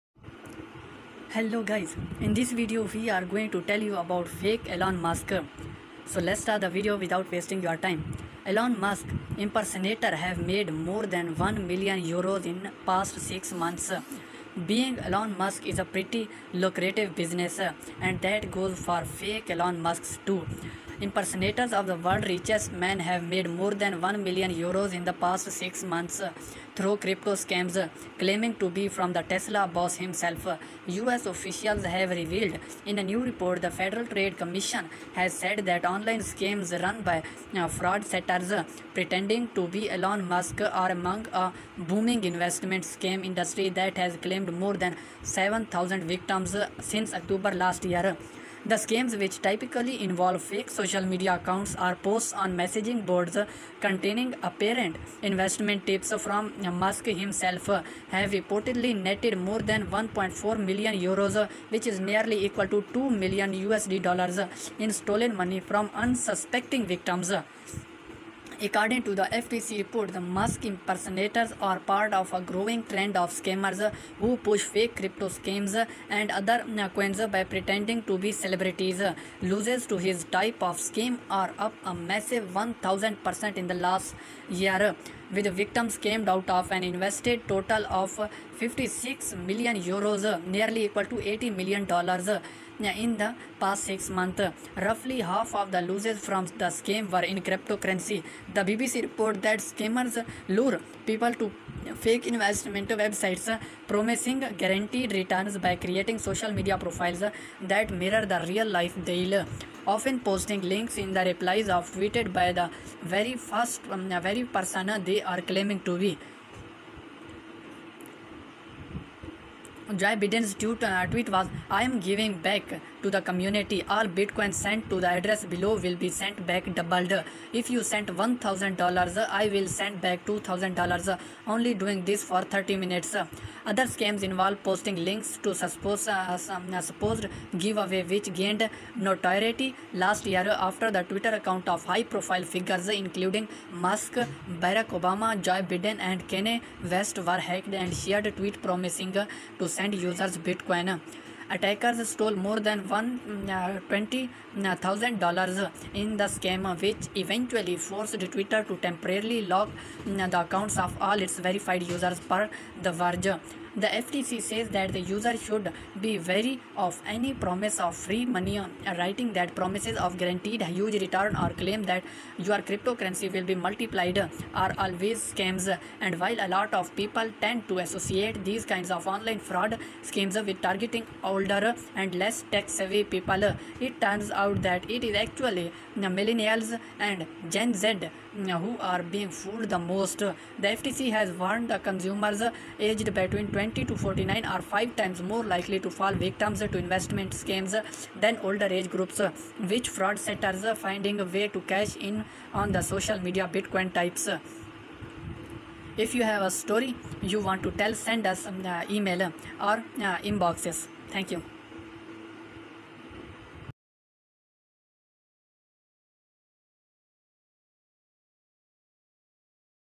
Elon Musk Impersonators Have Made More Than £1 Million In Past Six Months. || Fake Elon Musk
Being Elon Musk is a pretty lucrative business, and that goes for fake Elon Musks, too.
Impersonators of the world’s richest man have made more than £1 million in the past six months through crypto-scams claiming to be from the Tesla boss himself, US officials have revealed.
In a new report, the Federal Trade Commission has said that online scams run by fraudsters pretending to be Elon Musk are among a booming investment scam industry that has claimed more than 7,000 victims since October last year.